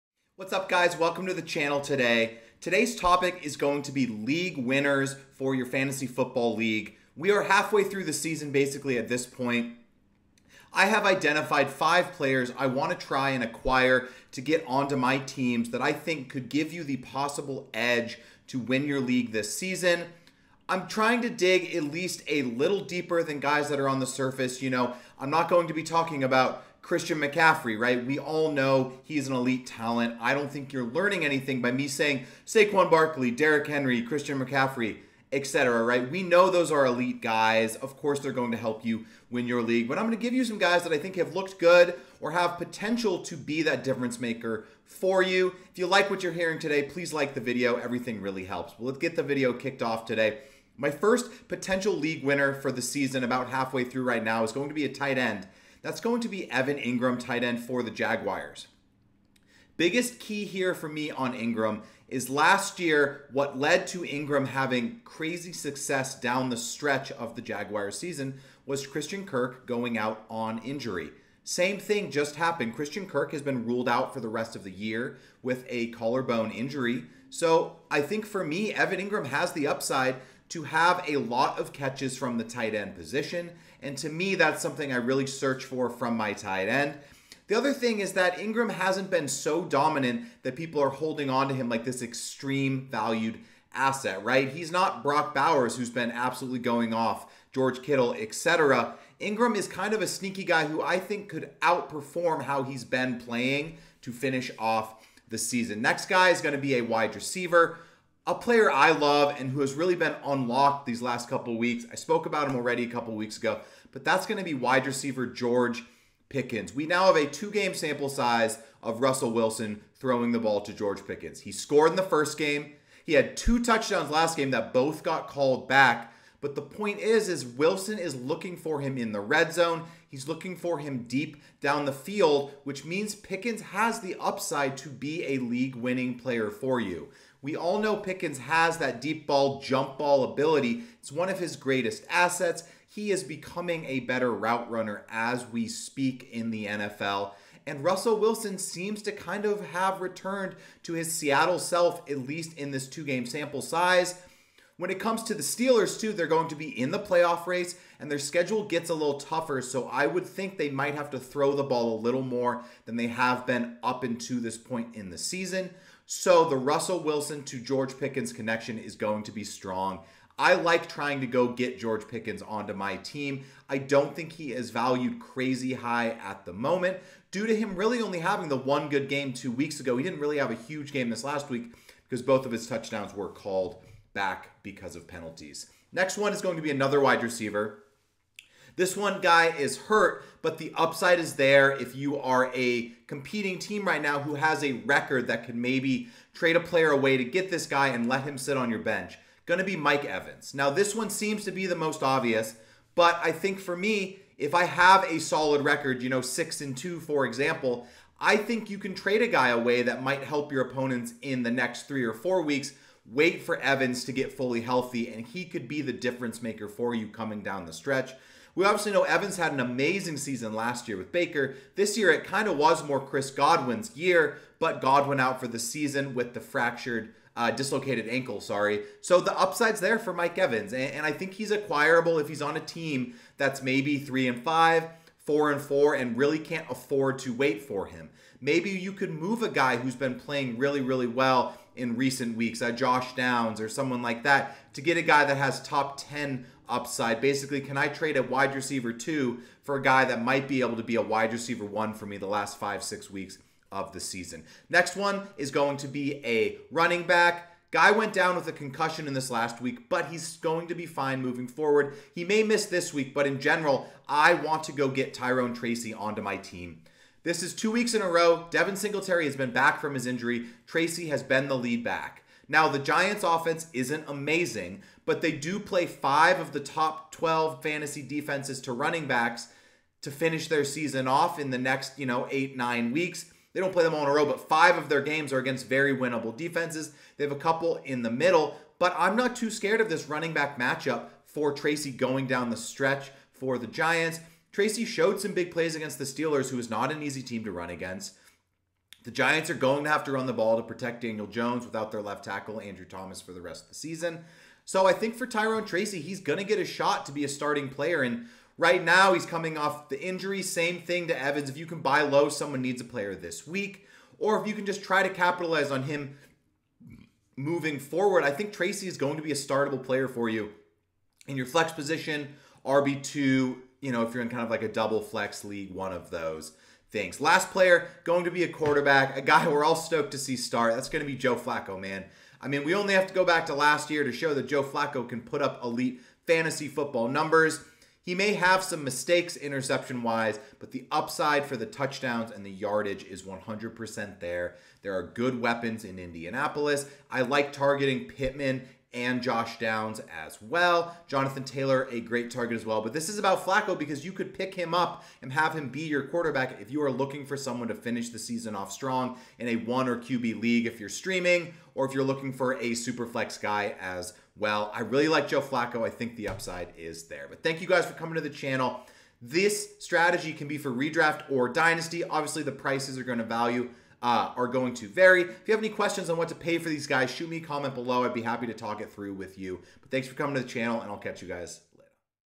TRADE FOR these 5 LEAGUE WINNERS in Fantasy Football!- Fantasy Football 2024
Go TRADE FOR these 5 Players RIGHT now IN Fantasy Football!

Timestamps:

00:00 Intro
00:57 Evan Engram
01:55 George Pickens
03:20 Mike Evans
04:25 Tyrone Tracy Jr.
05:31 Joe Flacco